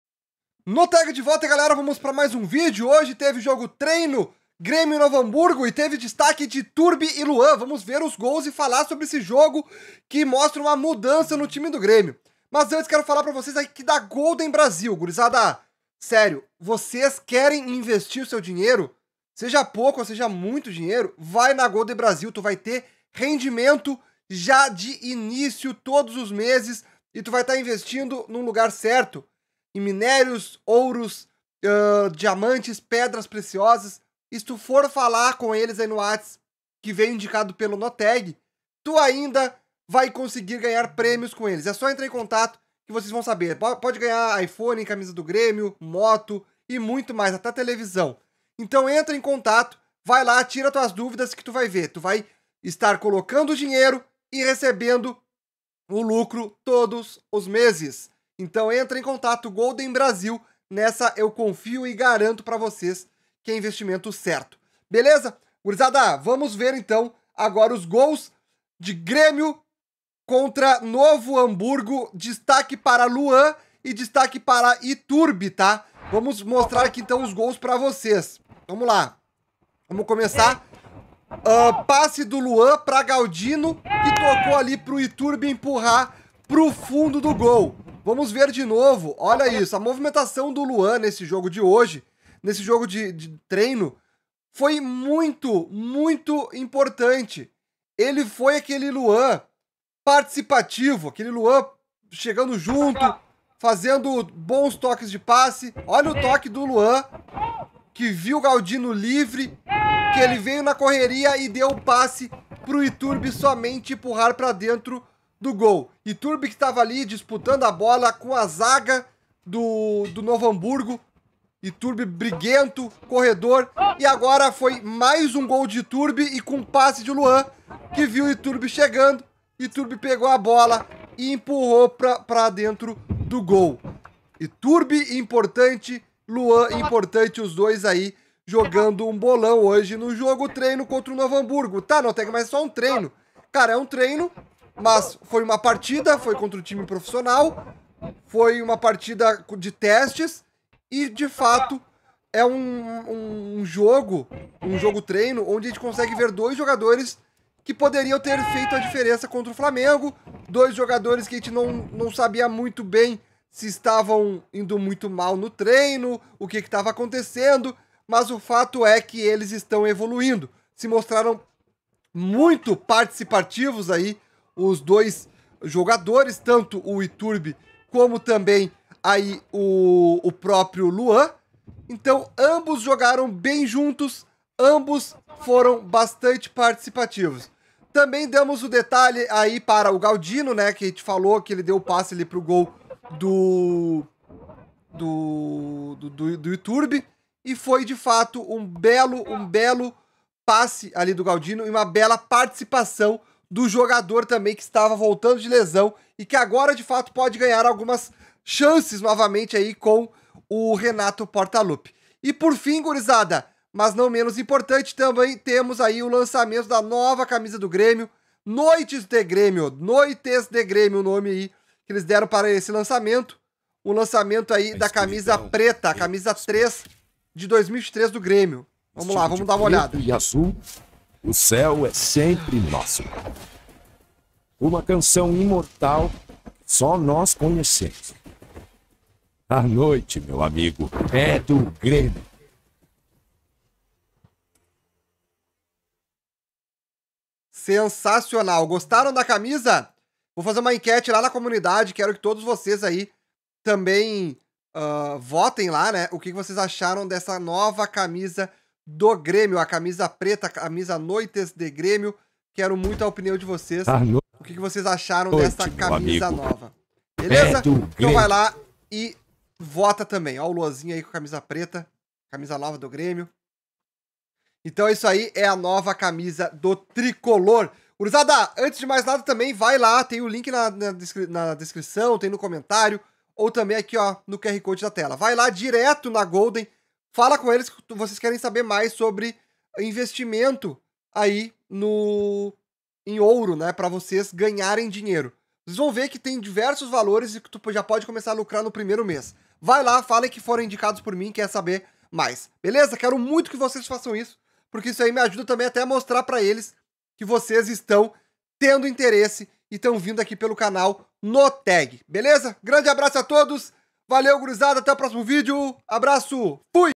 🚨 VEJA! ITURBE e LUAN JOGARAM MUITO HOJE NO JOGO TREINO DO GRÊMIO x NOVO HAMBURGO! GOL E ASSISTÊNCIA
👉Chamem eles no Whatsapp e falem que viram aqui no canal do NOTAG:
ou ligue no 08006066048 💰